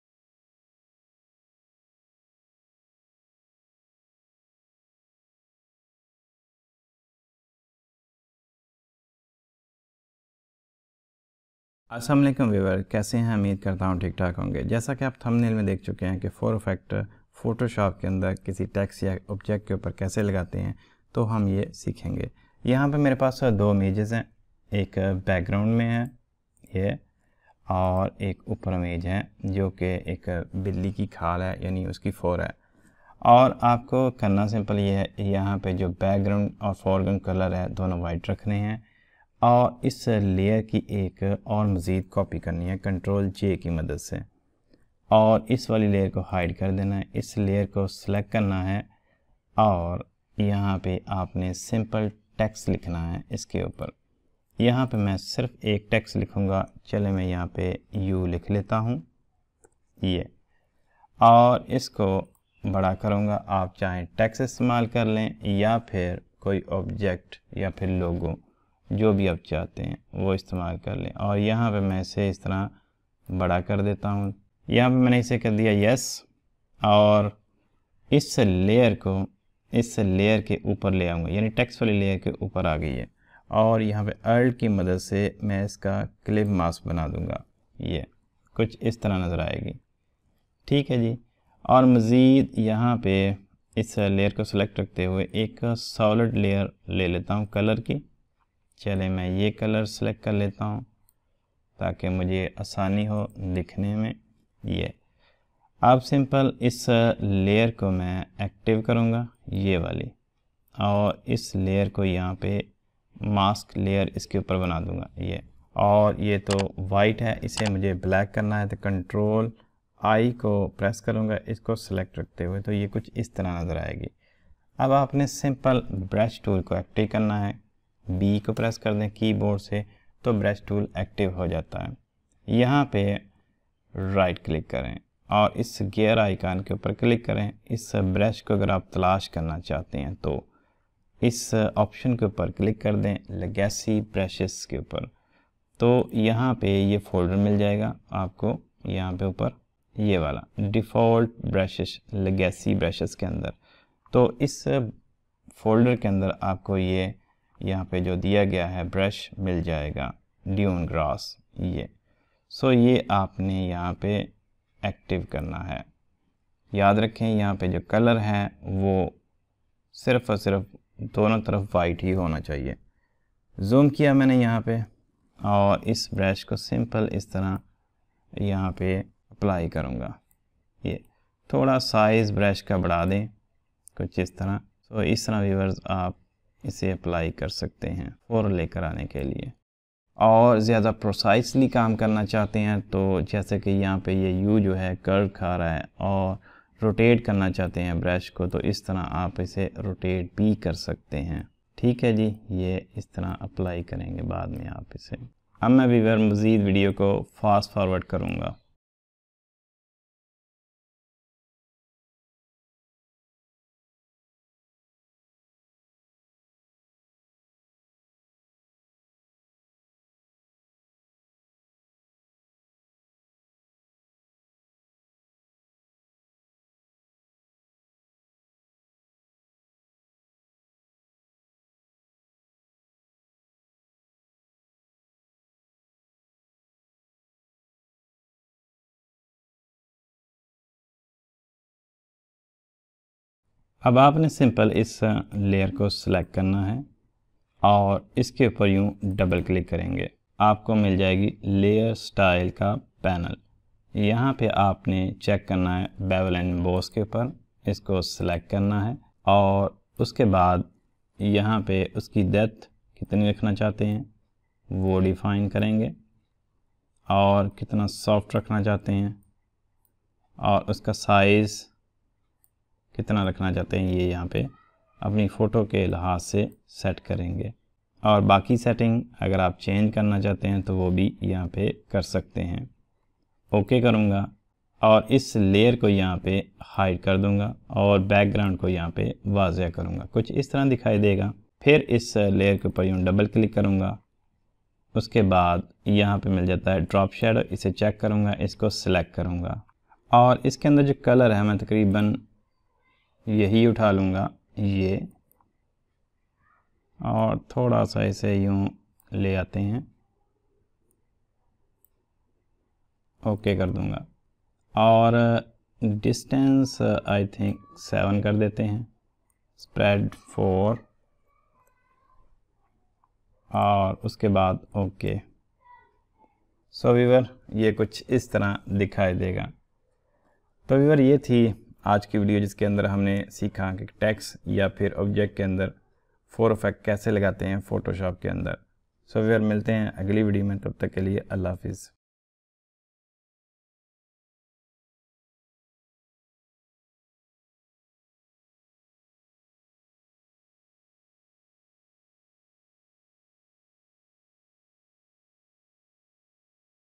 How to Make Fur Effect on Text or Object in Photoshop cc | Adobe Photoshop in Urdu & Hindi
Adobe Photoshop in Urdu, Photoshop in Hindi by Universe of Sarkar

Hi guys my name is Samiullah, welcome to Photoshop Training.
Now in this video we will learn how to make fur effect on text or object in adobe photoshop.

Universe of Sarkar

See other tutorials.